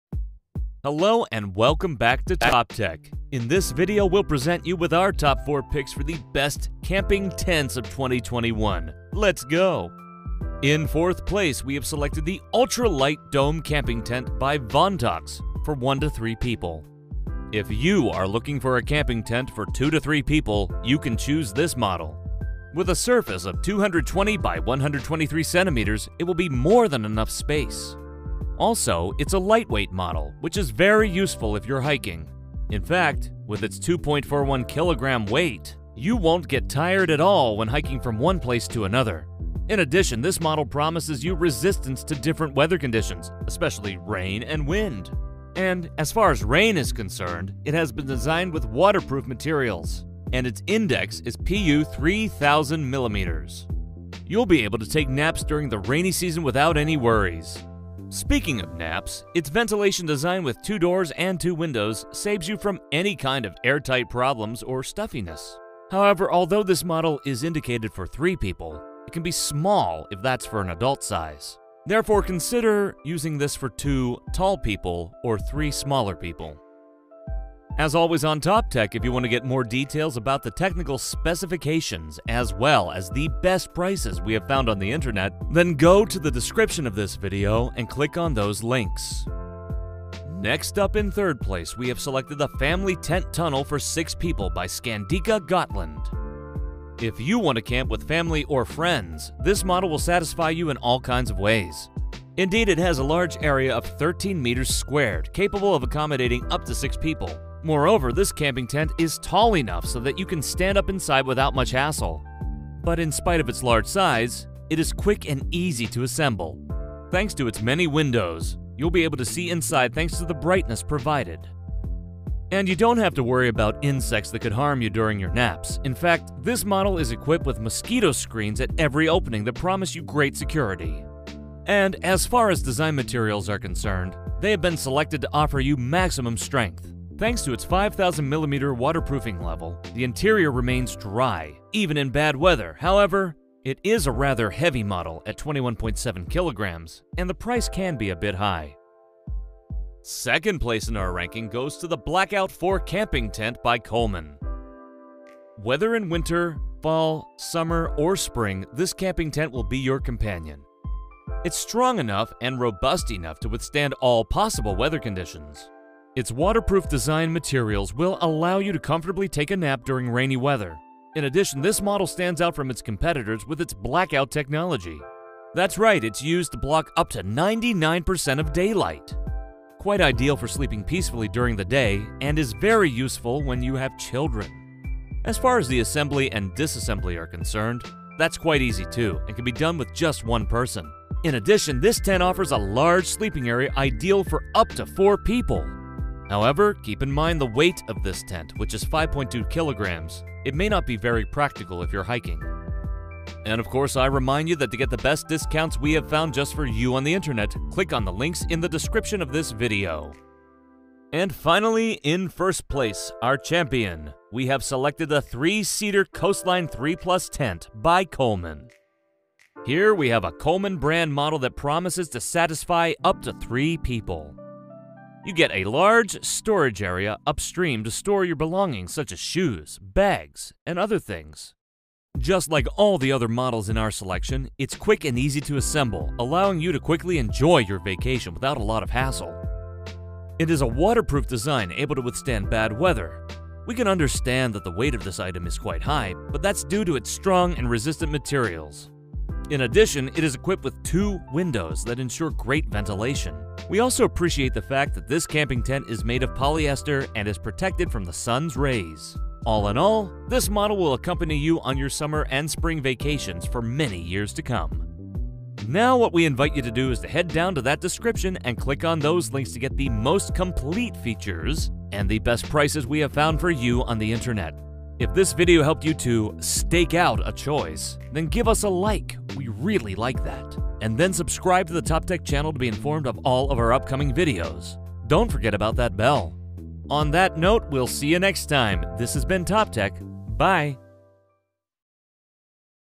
TOP 4: Best Camping Tent 2021
Best Camping Tent 2021, whether for backpacking or for a survival experience, here are the best tents (with room!) 2021 👇

👌🏼 All features and best prices here :
4 - Professional 4 in 1 Anti-Cellulite Massager Ardes - ARD.M240:
4 - Ultra Light Dome Camping Tent 1-3 people V VONTOX
3 - Family Tunnel Tent 6 people Skandika Gotland 6
2 - Coleman Blackout 4 Camping Tent
1 - Coleman Coastline 3 Plus 3-person tent